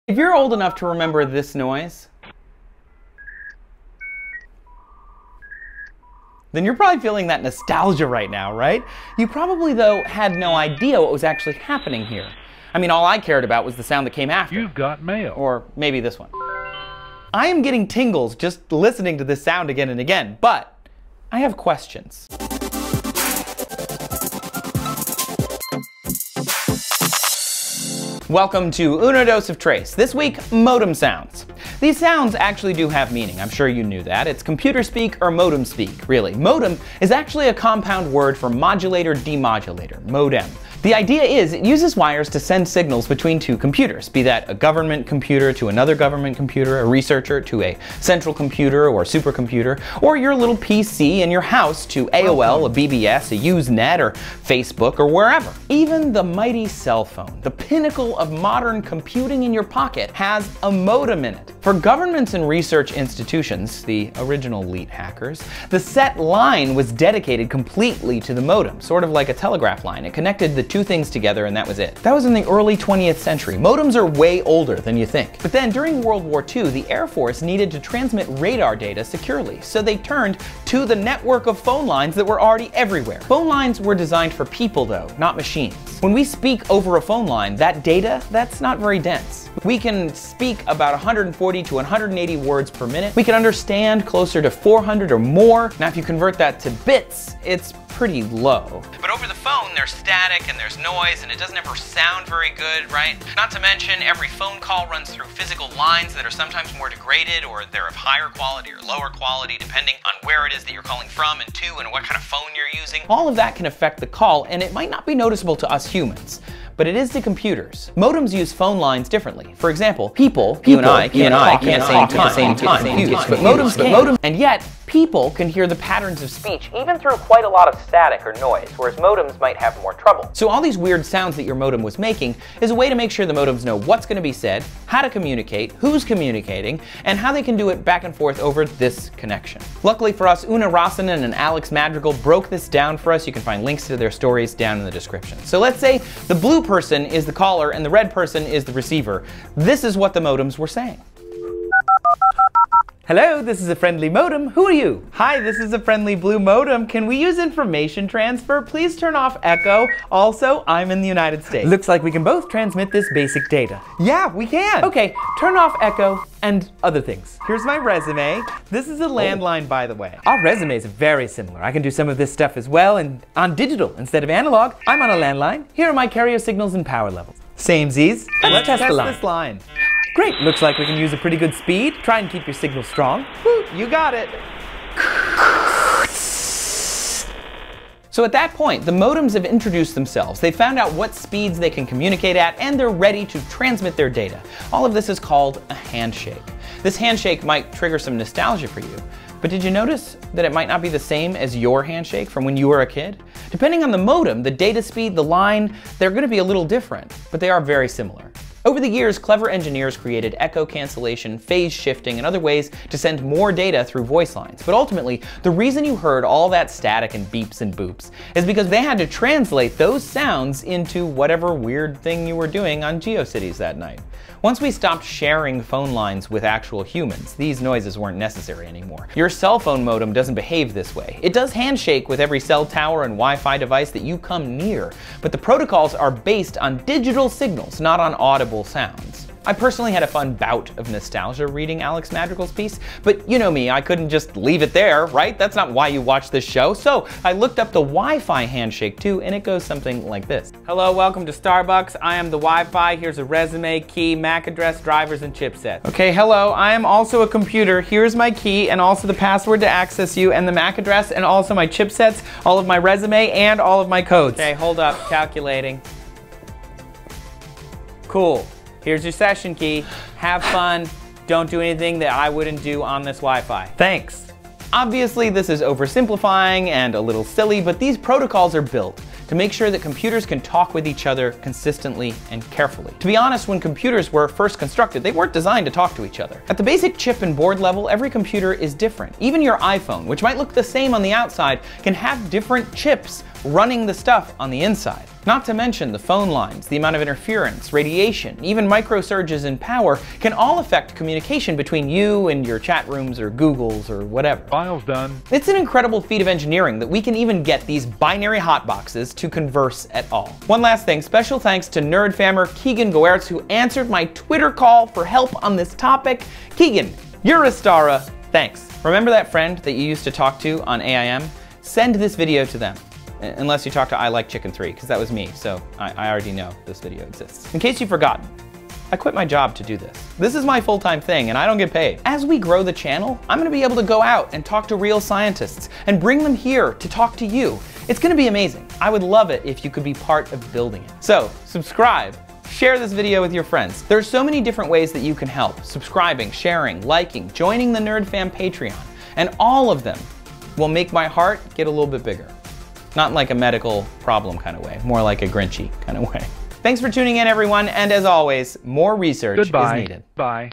Dial-Up Modems Made the Weirdest Sounds, Here's What They Actually Mean
Maybe you are old enough to remember dial-up internet, maybe you're not. || Watch this video on Nebula with no ads. Joining supports creators and me!

Chances are you've heard the droning wail of a modem as it attempts to connect to a far-off computerized confidant. What did all those beeps, boops, buzzes, and clangs mean? Luckily, I had questions.

~~~  Good Lynx  ~~~

These two pieces really informed this entire video, the first by Oona Räisänen is likely the most oft quoted piece on this topic that I could find across the whole internet! Check out her blog "Asorptions" while you're at it:

The second piece, by Alexis Madrigal at the Atlantic, is more of a curious exploration of the topic, but gets into more the fun and fascinating way to think about what these machines were doing 40+ years ago.

¡Come follow me on all those social machines!

Thumb photo by Tuur Tisseghem from Pexels

Thank you for reading the description, I hope you enjoyed this P.S. Easter Egg!

P.S - This science writer needed hearing aids, which amplify certain signals. Usually, we choose to amplify voices and vocal sounds, but in this case he asked if they could amplify WiFi! He ended up creating a soundscape for this ubiquitous technology. I really wanted to include this, but I simply ran out of time. Read all about it here: